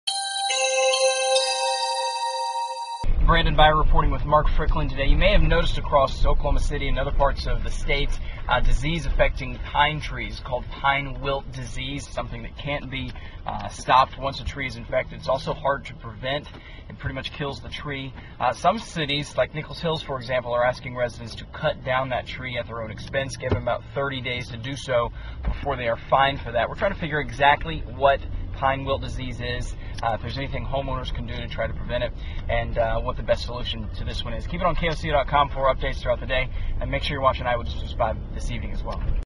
Disease Affects Pine Trees Across Oklahoma
Brandon Beyer is learning more about Pine Wilt disease. He'll have a full TV report on Eyewitness News 5 at 5 and 6 p.m. on Wednesday, including information on how homeowners can prevent the disease.